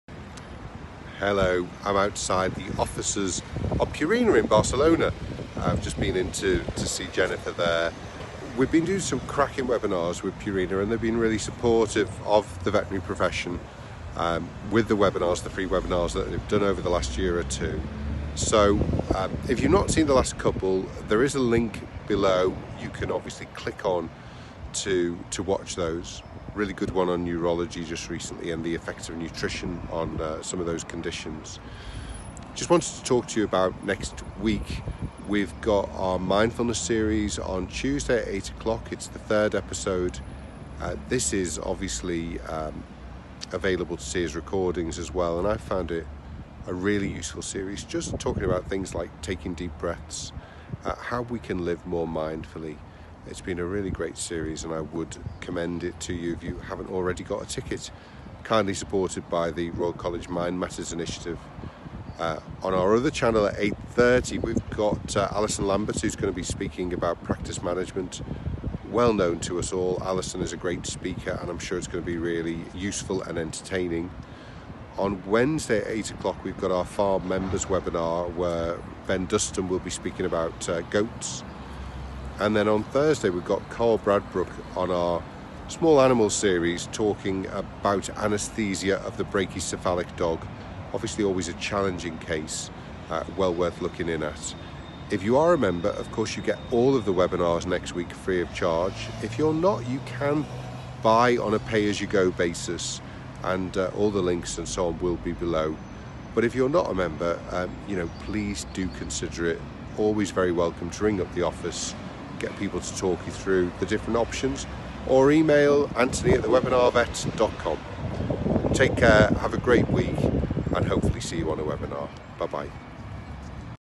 The Webinar Vet Week Ahead | 02/06/19
Find out what's coming up this week on The Webinar Vet with this week's The Week Ahead.

Catch up on our latest Purina webinars on "How Can Nutrition Impact the Brain?" and "Clinical and research experiences with probiotics" for FREE

For tickets to our Mindfulness course for just £40+VAT